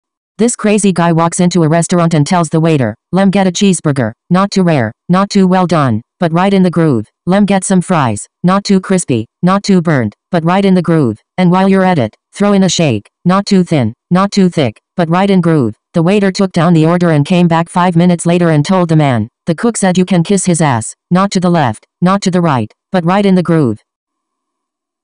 Jokes - This crazy guy walks into a restaurant and tells the waiter, “Lemme get a cheeseburger, not
Joke :
This crazy guy walks into a restaurant and tells the waiter, “Lemme get a cheeseburger, not too rare, not too well done, but right in the groove. Lemme get some fries, not too crispy, not too burnt, but right in the groove. And while you’re at it, throw in a shake, not too thin, not too thick, but right in groove.” The waiter took down the order and came back five minutes later and told the man, “The cook said you can kiss his ass, not to the left, not to the right, but right in the groove.”